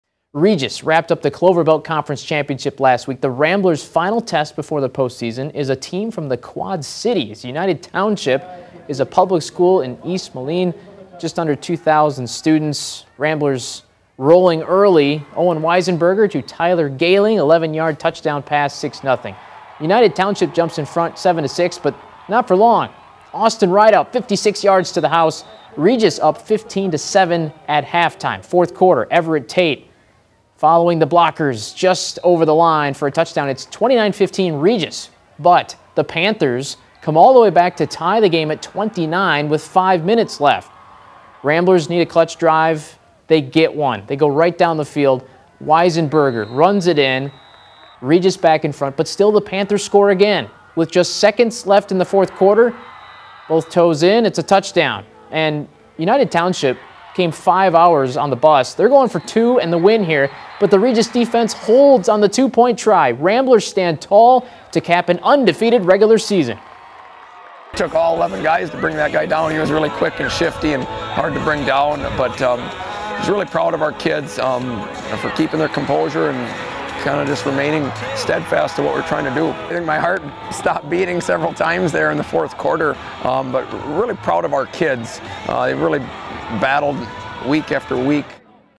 Ramblers defense stands tall late to secure undefeated regular season
Regis held off United Township, 36-35, thanks to a stop on a 2-point conversion in the final seconds Friday at Carson Park